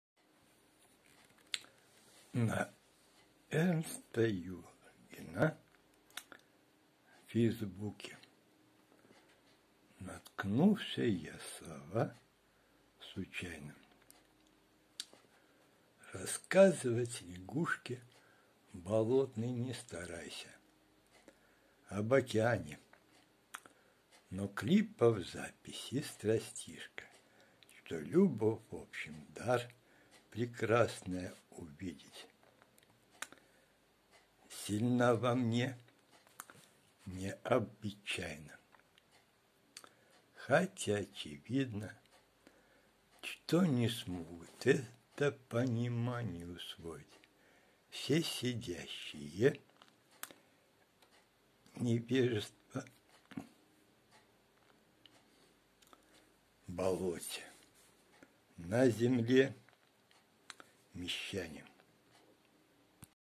Эрнста Юнгера слова
На Эрнста Юнгера в Фейсбуке наткнулся я слова случайно.
"Рассказывать лягушке болотной не старайся об океане".
Но клипов записи страстишка, что любовь, в общем,  — дар прекрасное увидеть, сильна во мне необычайно.
Хотя, очевидно, что не смогут это понимание усвоить все сидящие в невежества болоте на Земле мещане.